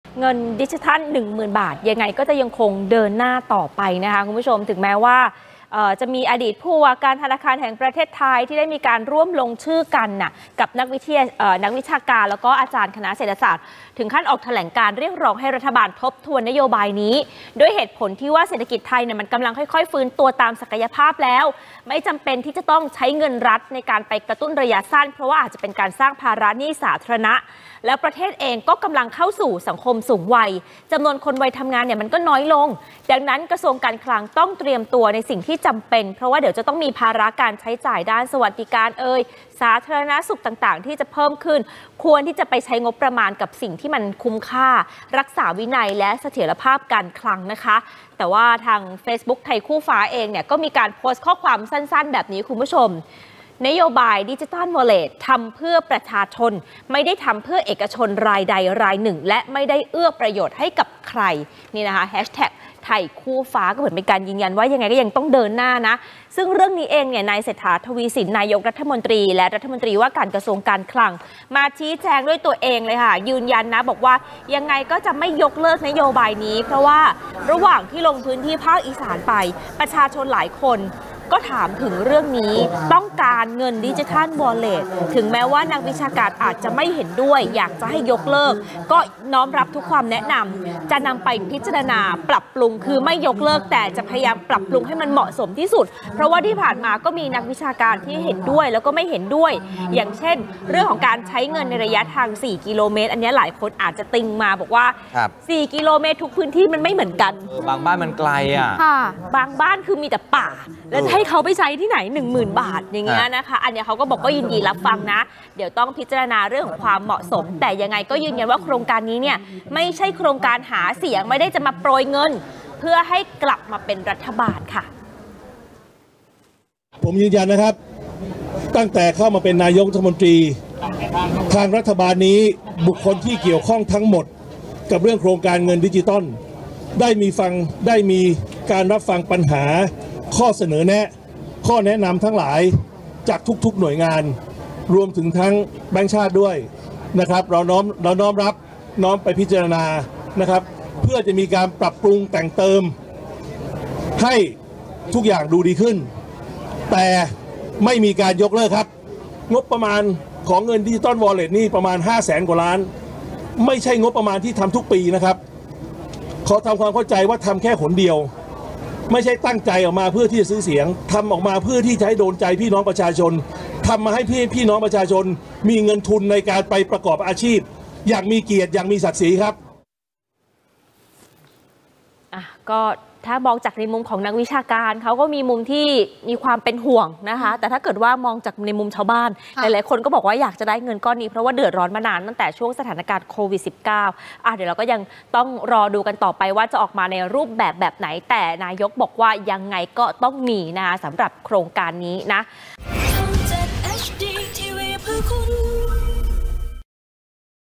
"เศรษฐา" ยืนกราน ! ไม่ยกเลิก แจกเงินดิจิทัล 10,000 บาท ยินดีรับคำแนะนำ
สนามข่าวเสาร์-อาทิตย์ - นายกรัฐมนตรี ยังคงเดินหน้าโครงการแจกเงินดิจิทัล 10,000 บาท แม้ว่าจะมีนักวิชาการ และนักเศรษฐศาสตร์บางคนออกมาต่อต้าน ยืนยันว่ารัฐบาลน้อมรับฟังทุกความคิดเห็น

อดีตผู้ว่าฯ ธปท. ค้าน รัฐบาลแจกเงินดิจิทัล 10,000 บาท
หลังจาก นายยวิรไท สันติประภพ และนางธาริษา วัฒนเกส สองอดีตผู้ว่าการธนาคารแห่งประเทศไทย ร่วมลงชื่อกับนักวิชาการและอาจารย์คณะเศรษฐศาสตร์ 99 คน ออกแถลงการณ์ เรียกร้องให้รัฐบาลทบทวน นโยบายแจกเงินดิจิทัล 10,000 บาท ด้วยเหตุผลที่ว่า เศรษฐกิจไทยกำลังค่อย ๆ ฟื้นตัวตามศักยภาพ จึงไม่จำเป็นต้องใช้เงินรัฐเพื่อกระตุ้นระยะสั่้น เพราะจะสร้างภาระหนี้สาธารณะ อีกทั้งประเทศกำลังเข้าสู่สังคมผู้สูงวัย จำนวนคนวัยทำงานน้อยลง ดังนั้นกระทรวงการคลัง ต้องเตรียมตัวในสิ่งที่จำเป็น เพราะจะมีภาระการใช้จ่ายด้านสวัสดิการ สาธารณสุขที่เพิ่มขึ้น ควรใช้งบประมาณที่คุ้มค่า รักษาวินัย เสถียรภาพการคลัง

ขณะที่เฟซบุ๊กไทยคู่ฟ้า ได้โพสต์ข้อความสั้น ๆ ว่า นโยบายดิจิทัลวอลเลต ทำเพื่อประชาชน ไม่ได้ทำเพื่อเอกชนรายใด รายหนึ่ง และไม่ได้เอื้อประโยชน์ให้กับใคร

เศรษฐา ยืนกรานไม่ยกเลิก แจกเงินดิจิทัล 10,000 บาท
แน่นอนว่า เรื่องนี้ นายเศรษฐา ทวีสิน นายกรัฐมนตรี และรัฐมนตรีว่าการกระทรวงการคลัง ต้องออกโรงชี้แจงด้วยตนเอง และยืนยันว่า จะไม่ยกเลิกนโยบายดังกล่าว เพราะระหว่างลงพื้นที่ภาคอีสาน มีประชาชนหลายคน ต้องการเงินดิจิทัลวอลเลต แม้นักวิชาการหลายคน ไม่เห็นด้วย และเรียกร้องให้ยกเลิก ก็ยินดีน้อมรับทุกคำแนะนำ ซึ่งจะนำไปพิจารณา ปรับปรุงให้เหมาะสม เพราะที่ผ่านมาก็มีนักวิชาการที่เห็นด้วย และไม่เห็นด้วย อย่างเช่น เรื่องการใช้เงิน ในระยะทาง 4 กิโลเมตร ก็มีหลายฝ่ายติงมาว่า บางพื้นที่มีแต่ป่า แต่เขาจะไม่สามารถใช้เงินได้ รัฐบาลก็ยินดีรับฟัง และกำลังพิจารณาความเหมาะสมอยู่ ยืนยันว่าโครงการนี้ ไม่ใช่โครงการหาเสียง และไม่ได้มาโปรยเงิน เพื่อหวังกลับมาเป็นรัฐบาลอีกครั้ง

ขณะที่ สส. ของพรรคเพื่อไทยหลายคน ได้รับการสอบถามจากประชาชนว่า เมื่อไหร่จะได้เงิน 10,000 บาท ซึ่งอยากให้คนคัดค้านรับฟังประชาชนที่หาเช้ากินค่ำบ้าง หรือเกษตรกร ที่ต่างต้องการความช่วยเหลือเป็นอย่างมาก จากปัญหาที่สะสมมาตลอดระยะเวลาหลายปี โดยเฉพาะในช่วงสถานการณ์การแพร่ระบาดโควิด-19 ที่ทำให้ชีวิตของประชาชนสะดุดจนติดลบ